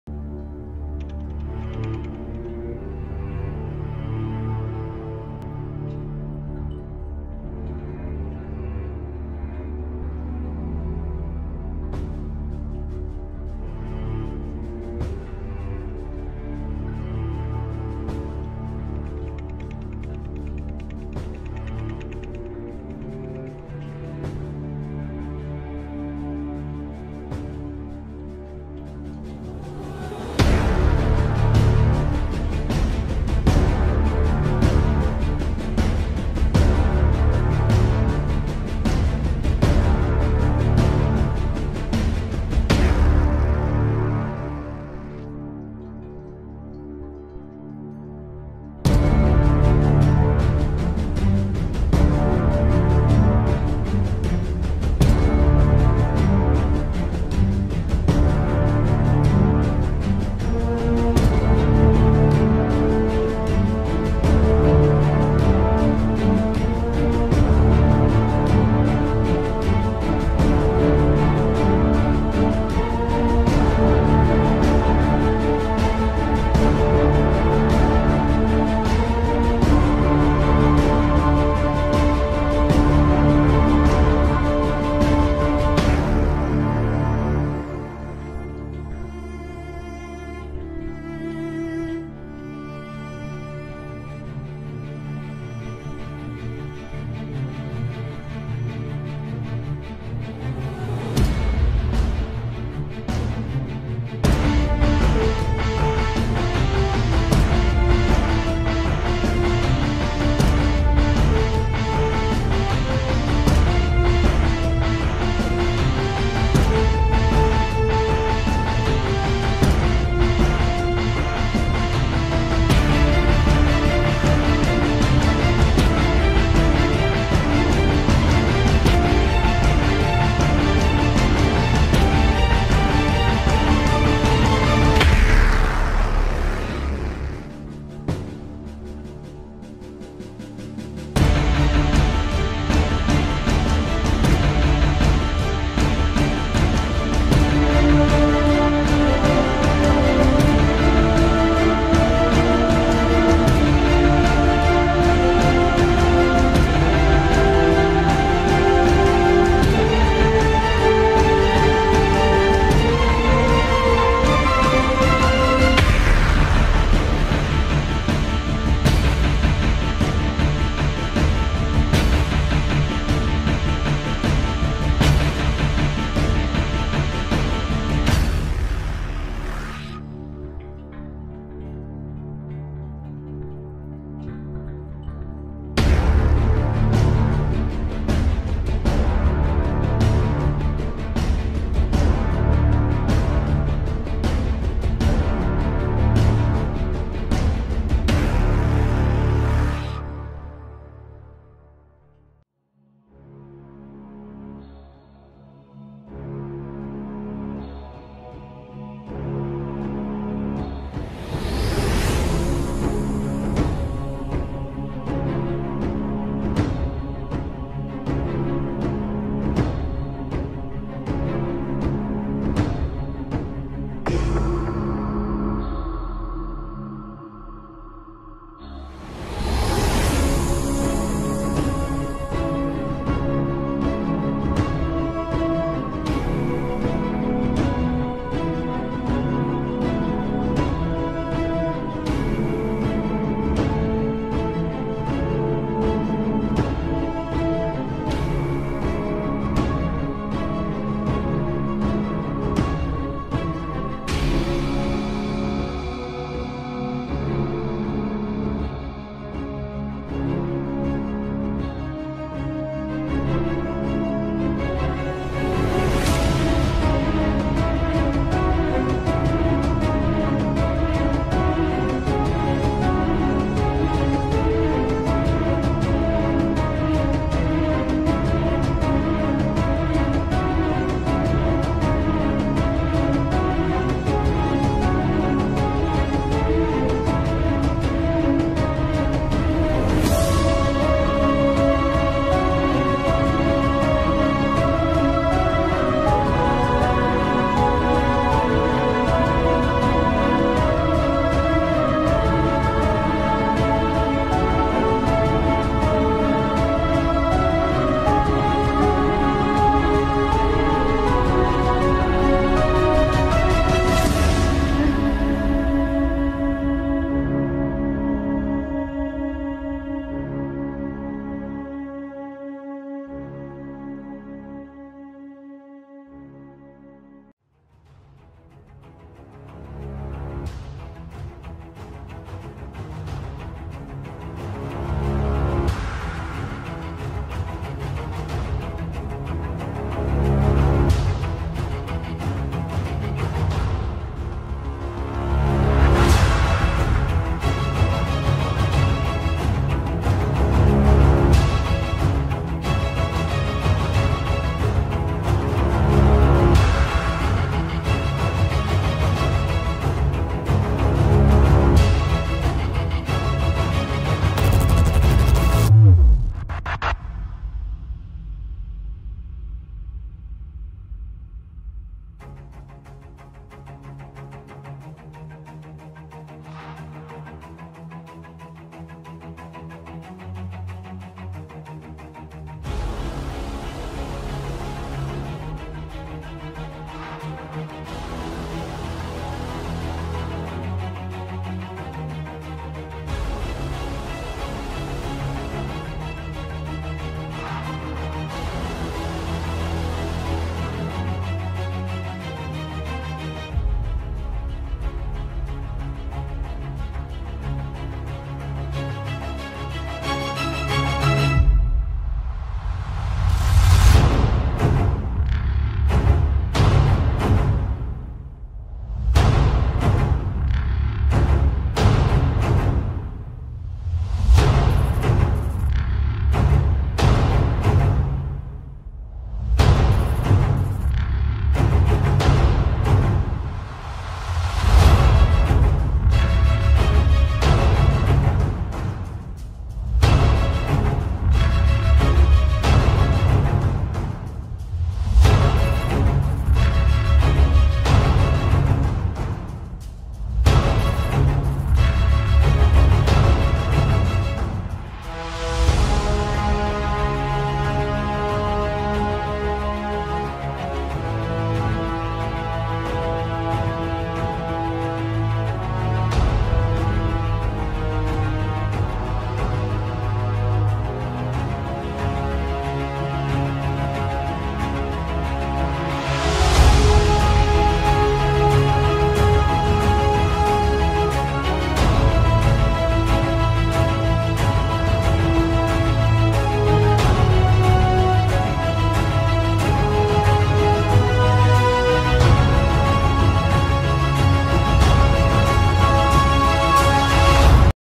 Painting Assassin's Creed Valhalla poster [Timelapse]
Painting Assassin's Creed Valhalla fan art. It was a tricky project that combined techniques to paint skin tones as well as drawing full figures.